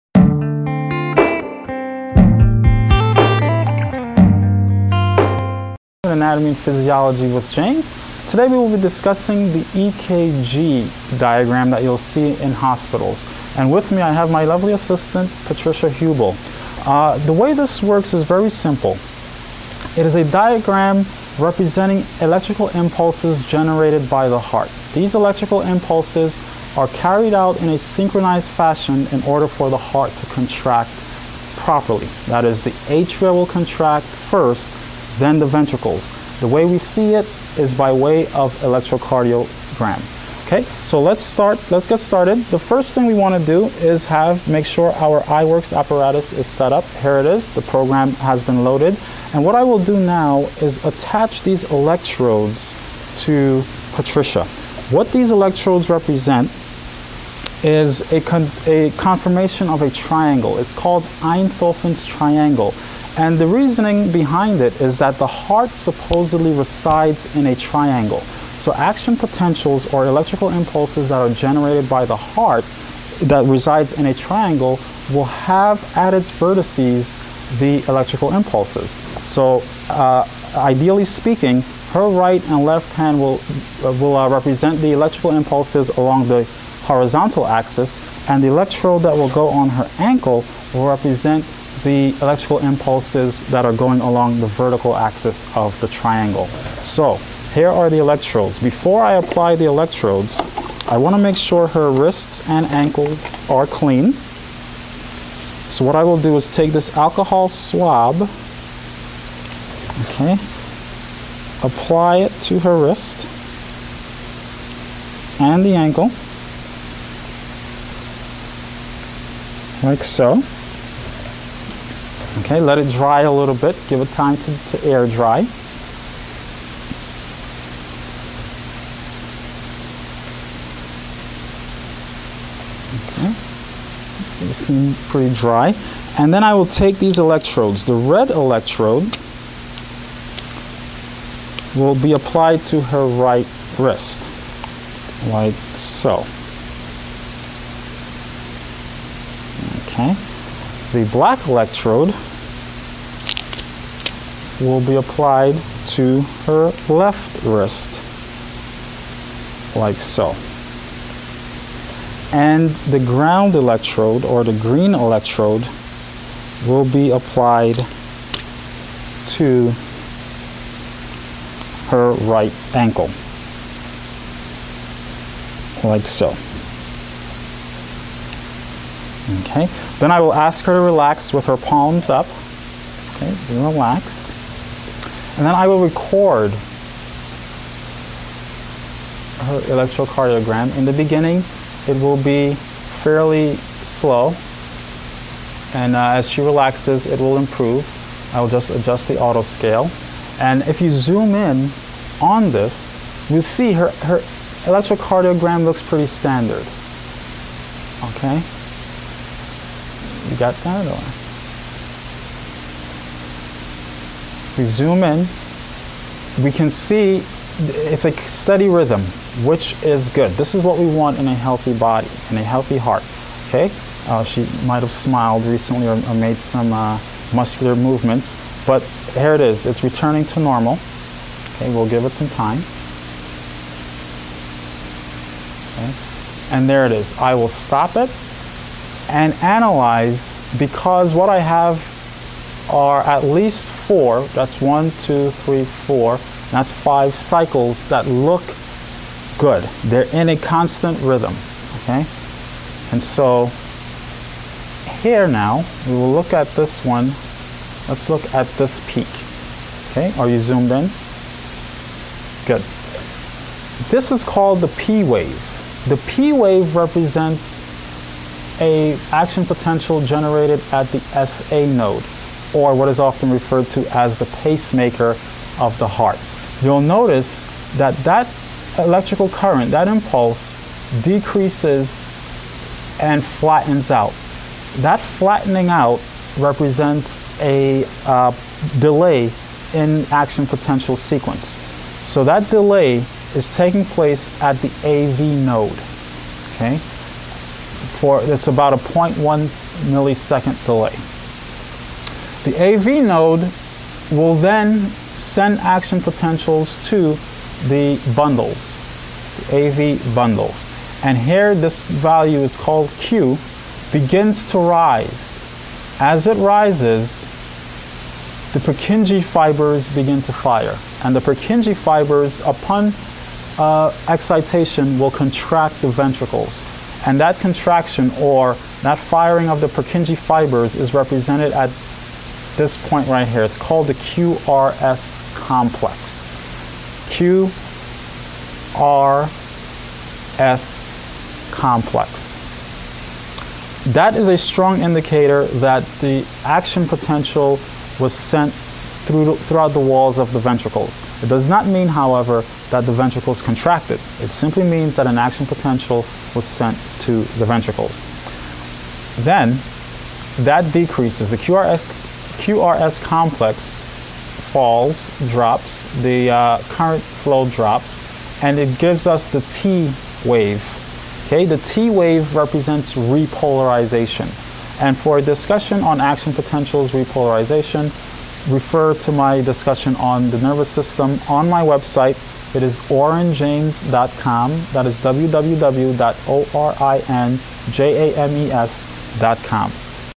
Reading an Electrocardiogram (EKG)
Using the iworx lab apparatus I read my assistant's (Patricia) electrogardiogram.